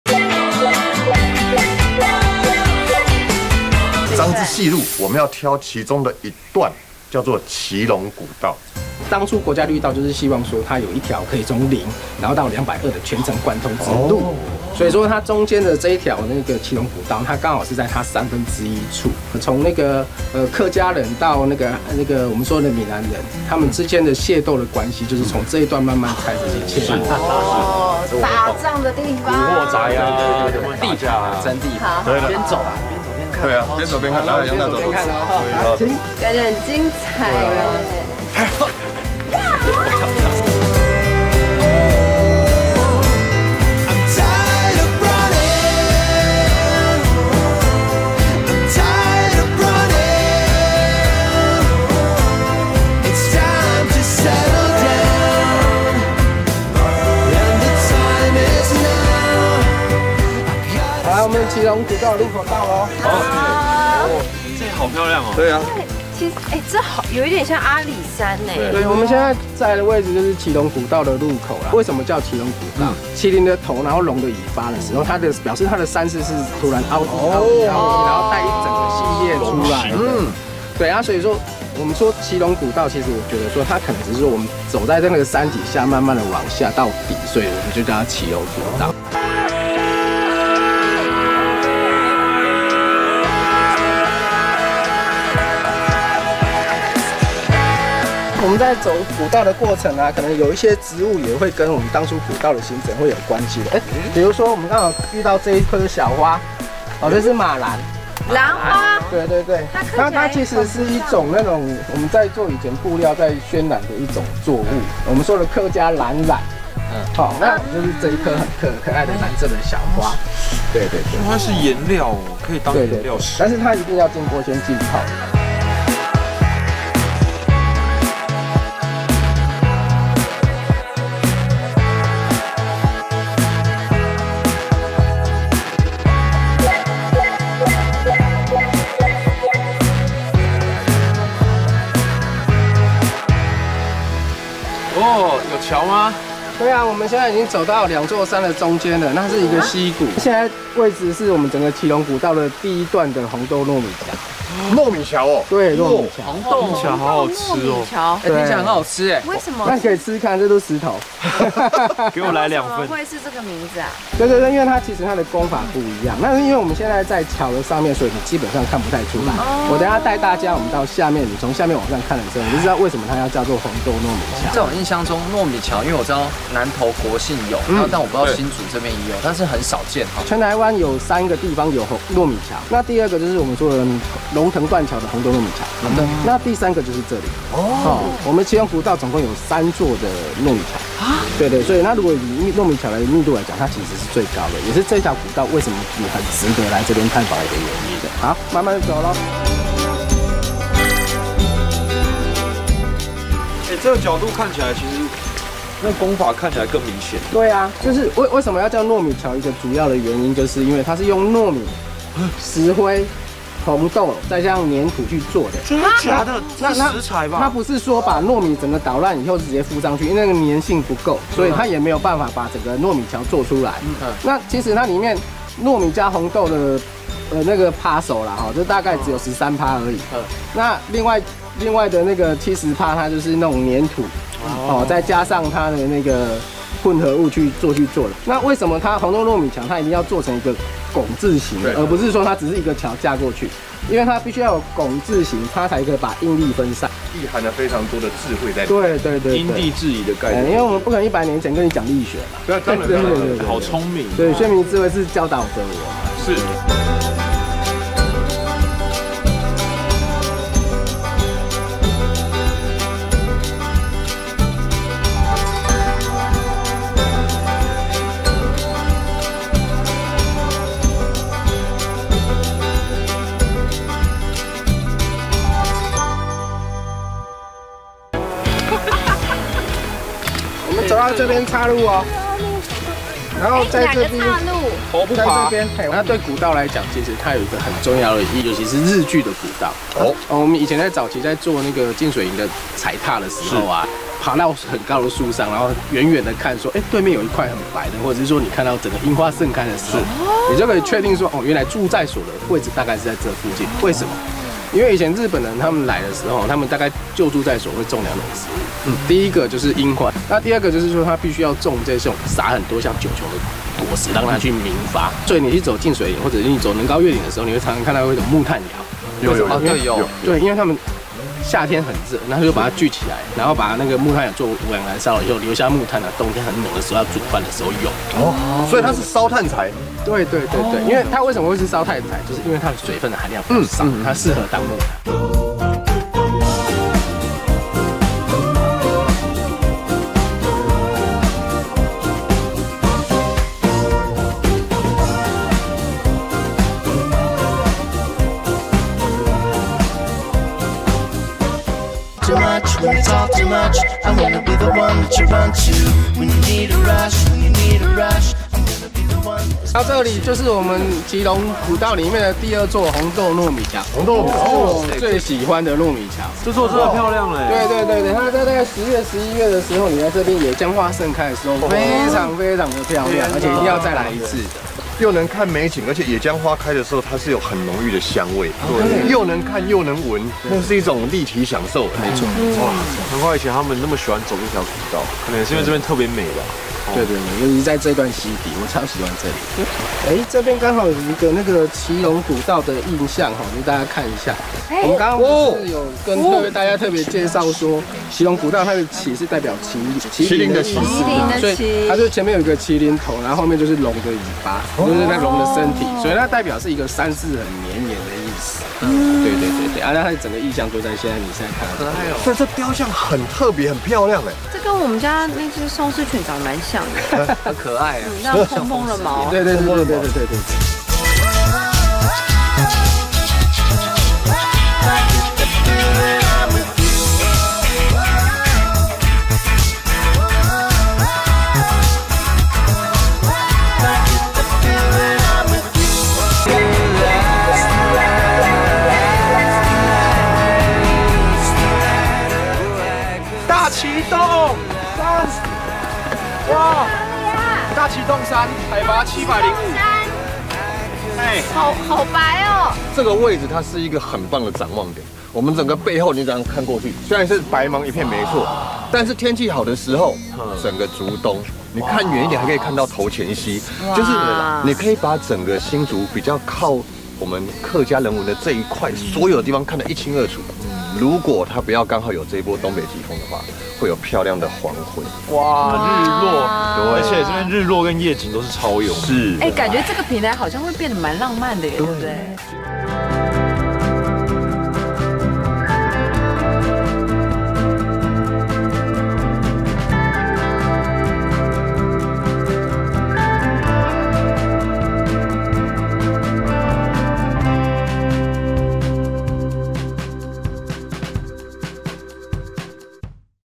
用紅豆和糯米做的橋？！全台只有三個地方有？！其中密度最高的就在新竹的騎龍古道！｜【上山下海過一夜】
快訂閱『上山下海過一夜』!